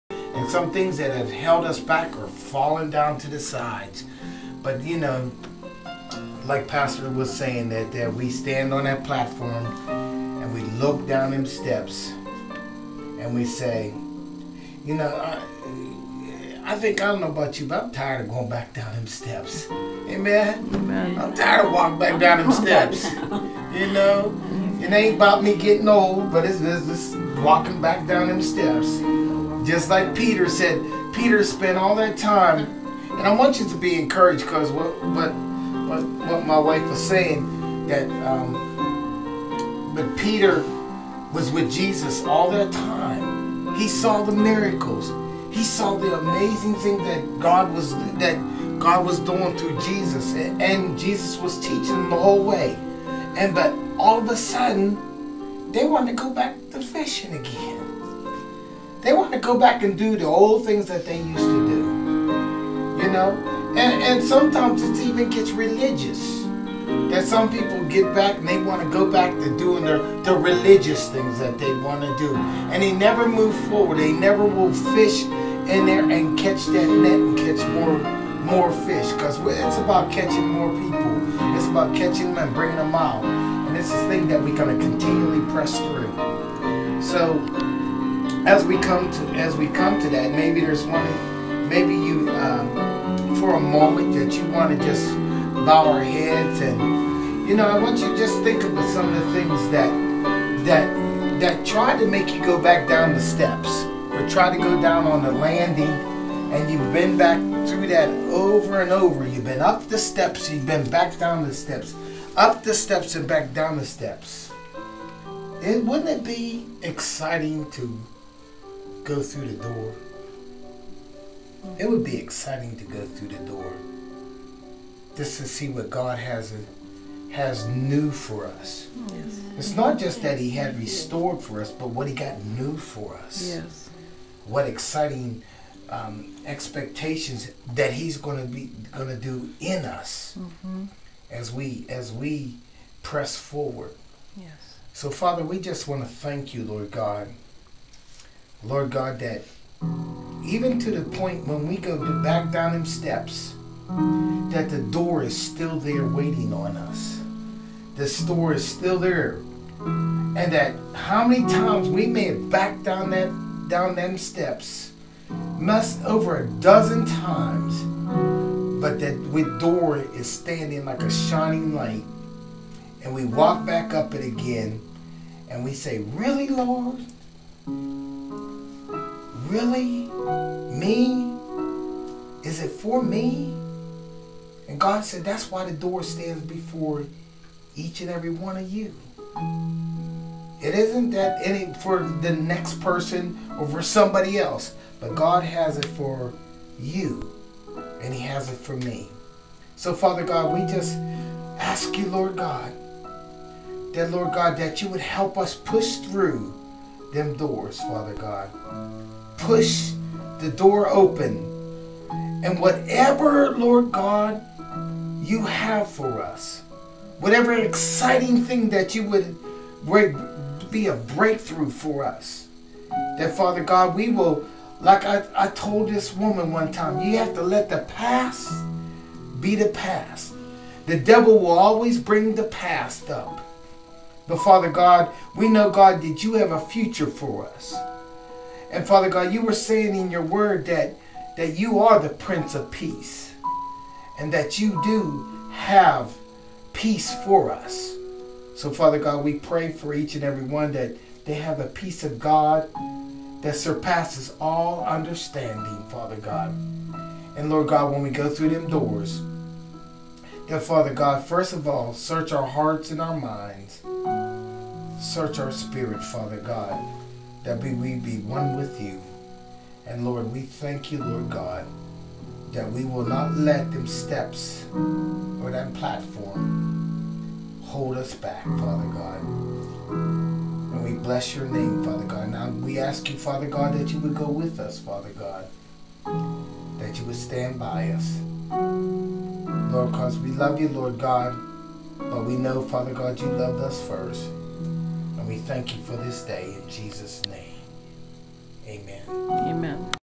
Undone Things Prayer & Declare
Pastor Walter recaps the word & prayer for you.